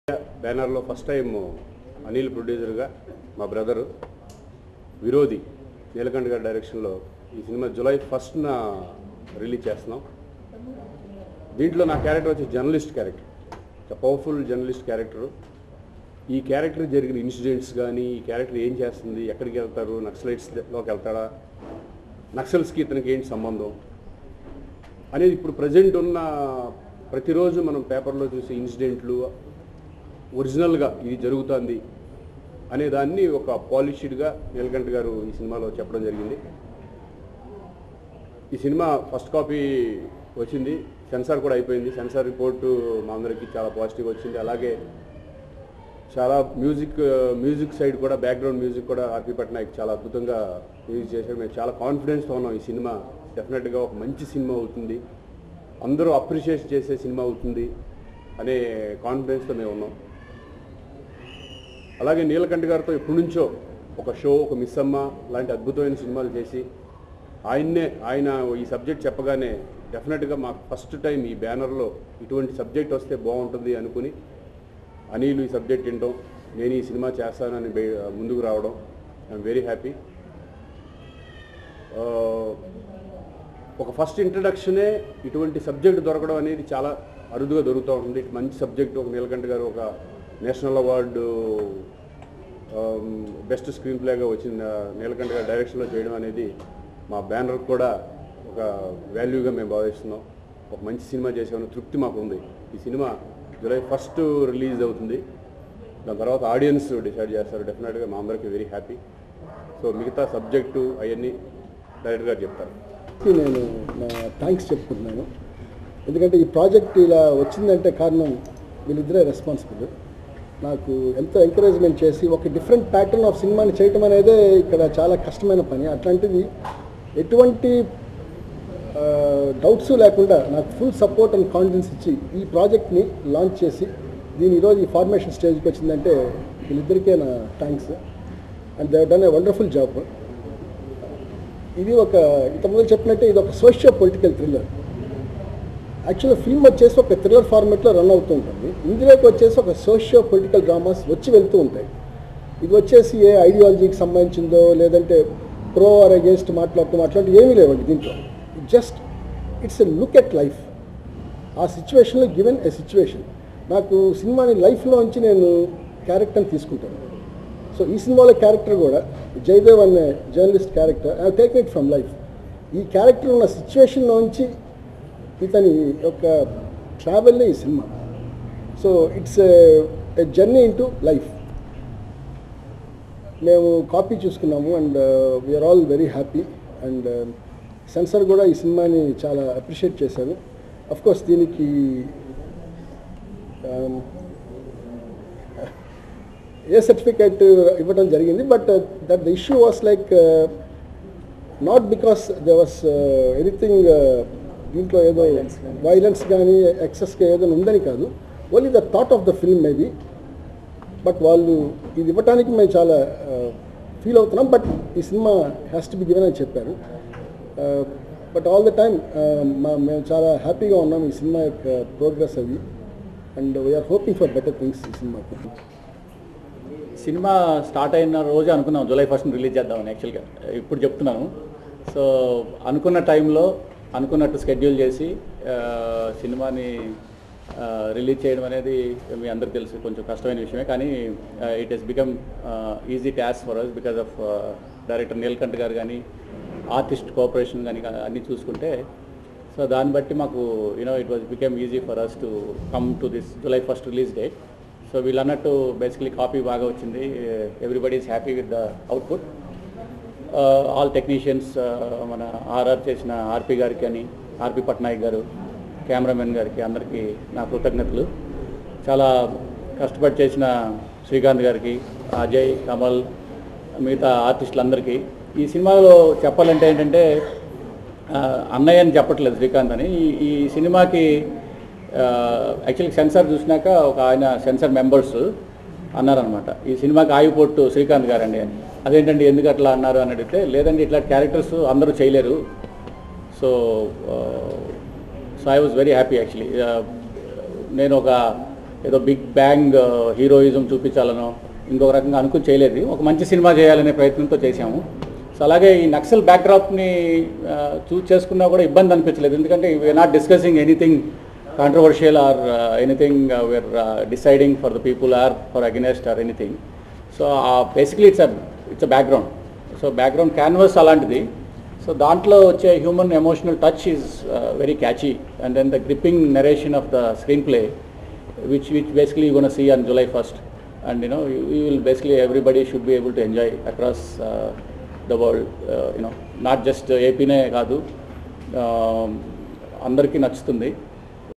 Virodhi Movie Press Meet- Srikanth, Neelakanta and Others
Virodhi Movie Press Meet, Hey see this Srikanth Latest Movie Virodhi Trailer Video. Its Neelakanta Movie and Its Cinejosh Videos